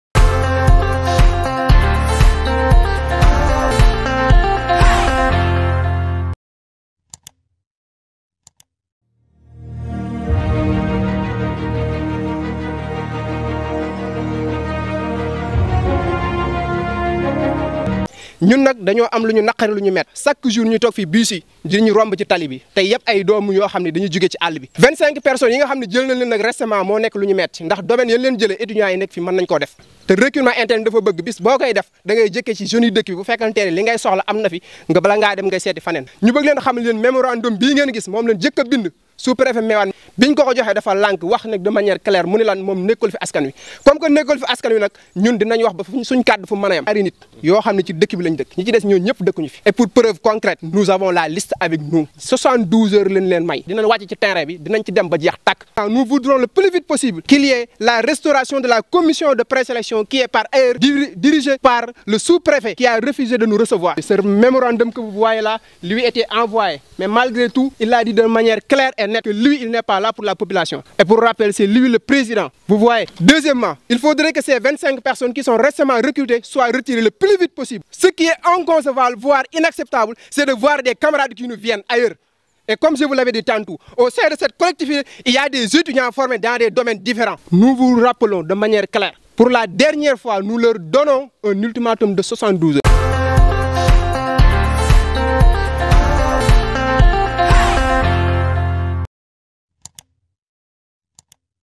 L'amicale des élèves et étudiants de Diogo. Dénoncé le recrutement de 25 personnes par la GCO dont .
L'amicale des élèves et étudiants de Diogo. Dénoncé le recrutement de 25 personnes par la GCO dont une seul habite a Diogo alors que les critères sur lesquelles elles ont été sélectionnées , existent au niveau locale. L'amicale exige l'annulation du recrutement de ces 25 personnes, la sélection des. Jeunes qui remplissent les critères requis pour respecter le volet interne du recrutement, la restauration de la commission de pré-selection au niveau de la zone de Diogo. L'amicale des jeunes élèves et étudiants de Diogo accordé un délais s de 72 h aux autorités de la GCO pour rectifier le tir sans quoi, elle va passer à la vitesse supérieure. En outre ces jeunes de Diogo regrettent la réaction du préfet qui refusent de les rencontrer pour recevoir leur mémorandum. Pis, selon ces jeunes le sous- préfet leur a carrément signifié qu'il n'était pas là pour défendre les intérêts des populations. Ce qui, à leurs yeux est inadmissible.diogo cris de coeur marche#